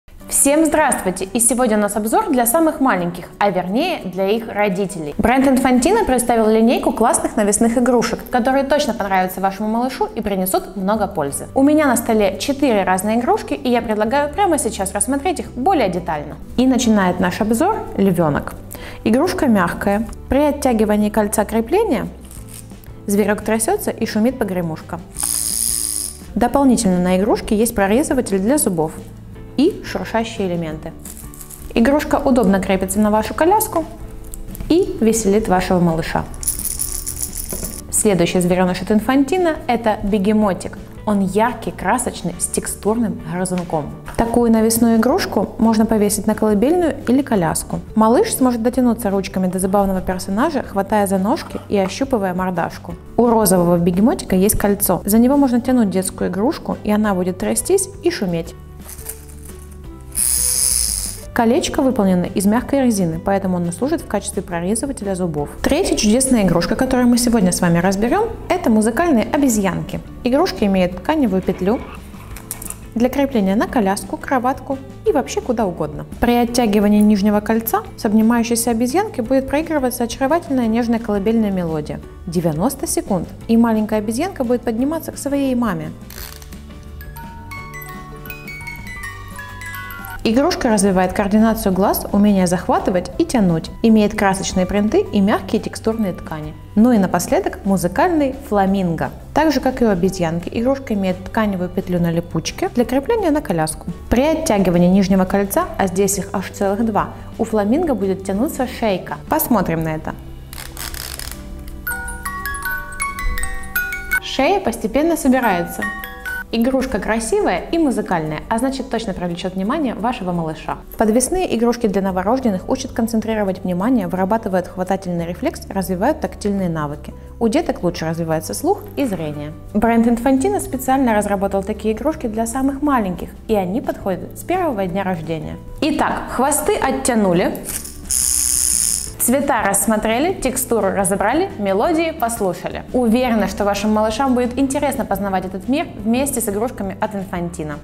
INFANTINO Навісна м'яка іграшка з прорезивателей "Бегемотик"
Навісну м'яку іграшку з прорізувачем "Бегемотик" можна повісити на колискову, коляску. Малюк зможе дотягнутися ручками до забавного персонажа, хапаючи за ніжки і обмацуючи мордочку. У рожевого бегемотика є кільце. За нього можна тягнути дитячу іграшку. Колечко виконано з м'якої гуми, тому воно служить в якості прорізувача зубів. Підвісні іграшки для новонароджених вчать концентрувати увагу, виробляють хапальний рефлекс, розвивають тактильні навички. У діток краще розвивається слух і зір.